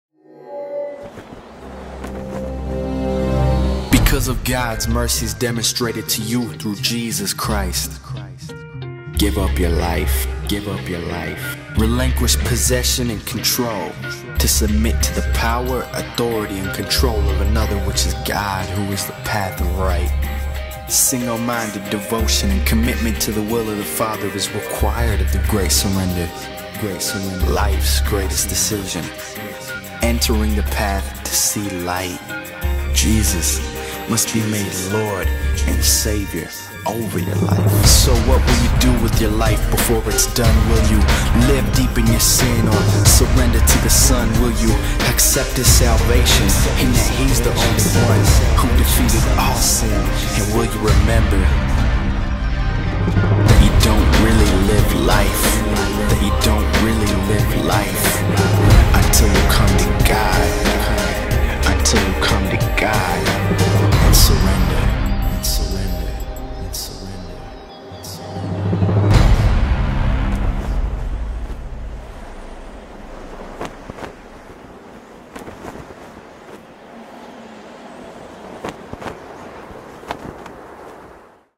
YC Manitoba 2012 "Surrender" Trailer HD
HD Trailer for YCMB12 "Surrender" - YC Manitoba takes place in Winnipeg, Manitoba on April 27-29, 2012. Check out YC.extremedream.ca for more info.